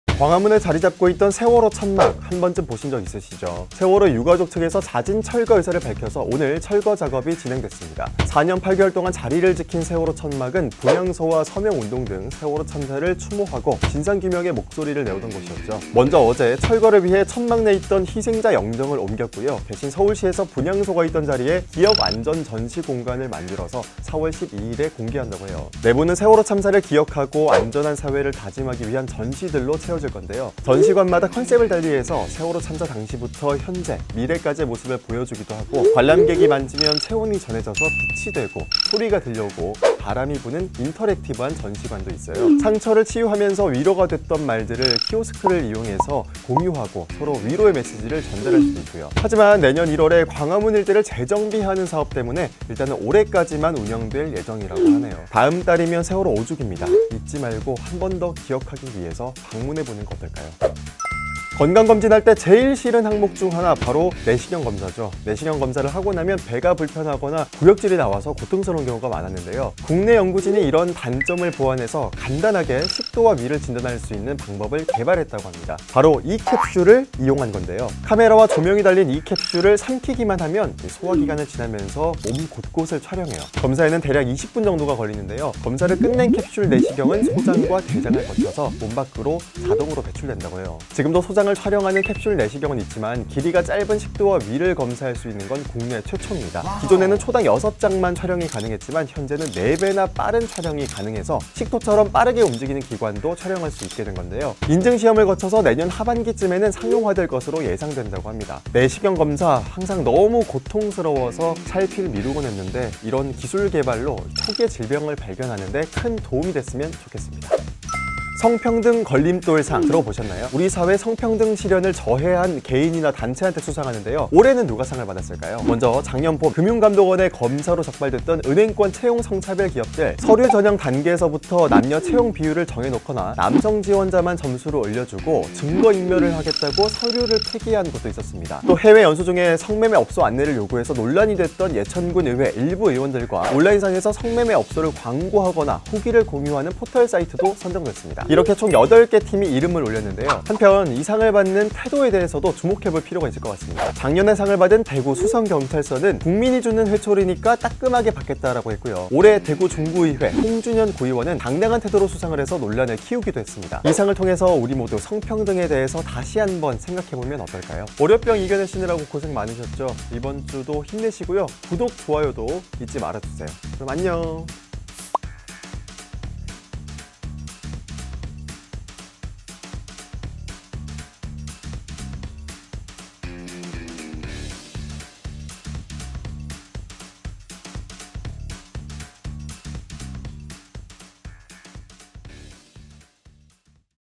세월호 천막 대신 재탄생할 기억안전공간 / 14F
광화문에 자리 잡고 있던 세월호 천막이 유가족 측에서 자진 철거 의사를 밝혀서 오늘 철거됐습니다. 4년 8개월 동안 분향소, 서명 운동 등 참사를 추모하고 진상 규명에 목소리를 내오던 곳이었죠. 이곳은 새롭게 기억.안전 전시공간으로 재탄생해 4월 12일에 공개된다고 하는데요. 안전한 사회를 다짐하는 전시로 채워질 거라고 합니다. 5주기를 앞두고 어떤 전시 공간이 생길지 알려드릴게요.